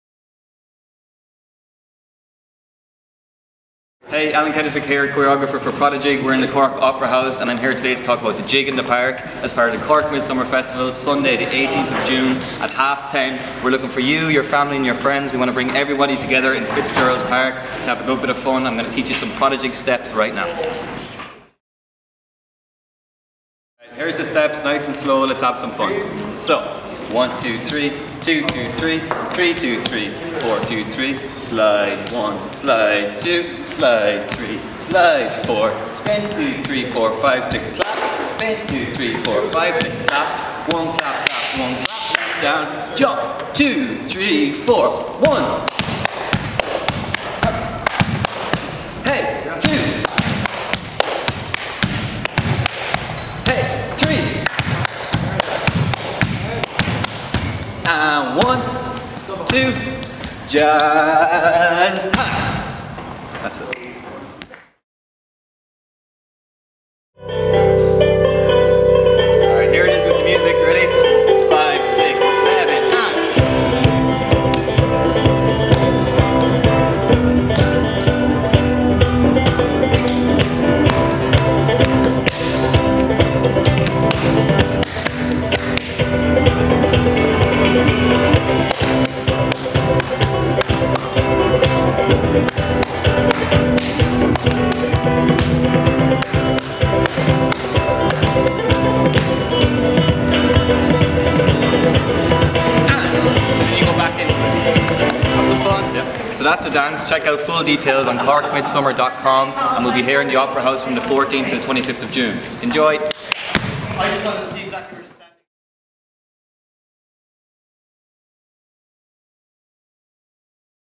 The Jig In The Park with ProdiJIG!
Grab your dancing shoes, and get learning some steps with ProdiJIG!!

To celebrate the magic of Midsummer, ProdiJIG and Cork Opera House are thrilled to be partnering with Cork Midsummer Festival to bring you The Jig In The Park! In keeping with the spirit of Midsummer, Cork Midsummer Festival will be staging Ireland’s largest ever Maypole Dance - an iconic Midsummer ritual, this year inspired by the sensational dance show ProdiJIG: The Revolution.

Alan Kenefick and ProdiJIG will be on hand to lead us all in a fun, specially created, once off Cork Midsummer Jig!

So we need YOU, your family and friends, and anybody else you can grab, to come down to Fitzgerald Park on the morning of June 18th to help us create another very special Midsummer moment!

So have a look at these steps and simply turn up on the day and pick up the steps as you go from the other dancers.

What you need: White clothing (if possible), a strong sense of fun!

Get Your JIG On!!!